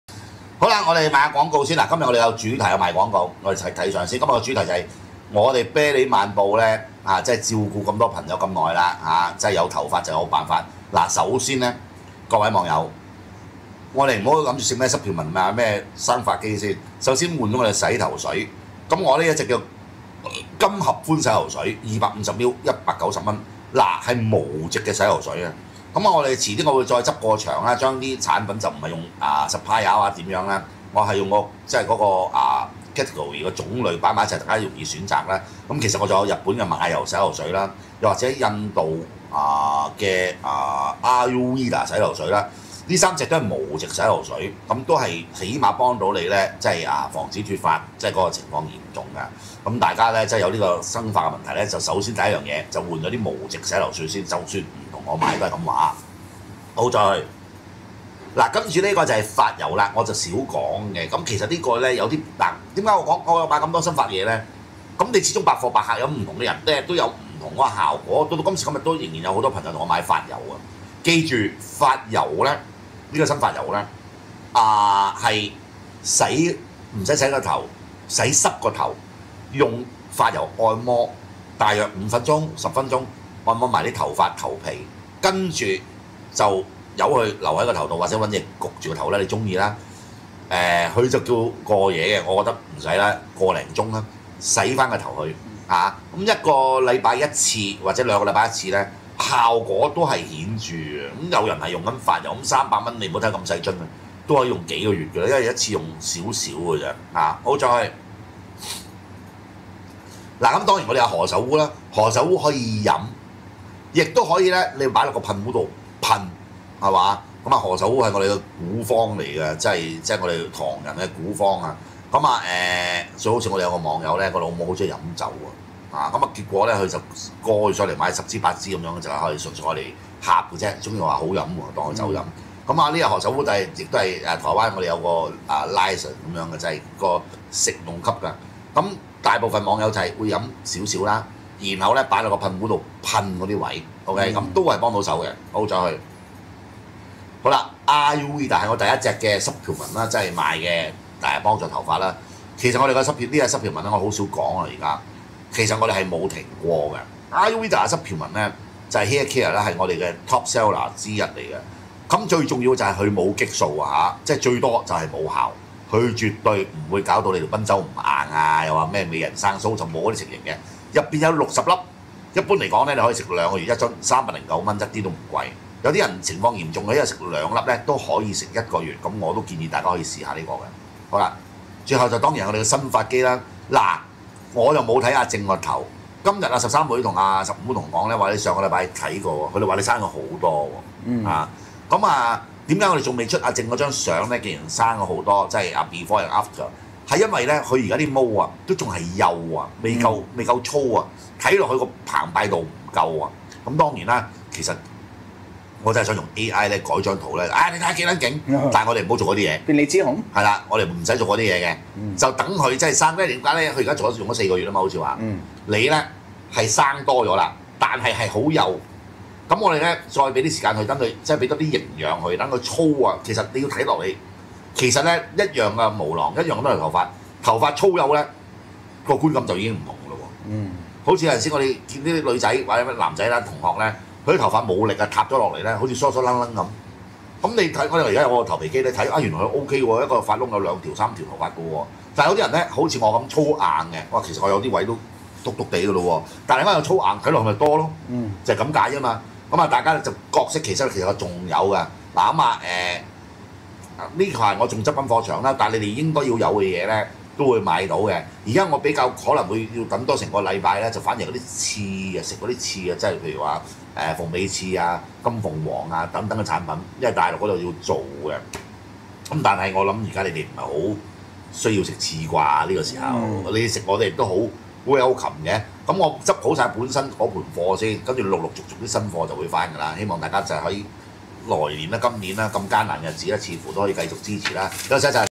20240228 啤梨晚報撚屌馬健賢又推好耐無推啲鳩吹可以生髮洗頭水等，仲吹遲啲會有好多新款貨到，又要等佢執好個貨架先。個貨架由上年年中執到而家，仲成日自讚自己好整齊好叻執貨架，偷錢賊咪影咗出嚟囉
又吹遲啲啲翅會到。
根本仲係滯銷賣唔去，繼續雪，端午節又鳩吹係新返貨再推鳩叫腦殘買嚟做節。
呢段日子咪貼個新到期日，其至貼過另一款面紙扮新一批貨囉
撚屌話雪著擺廿年都仲食得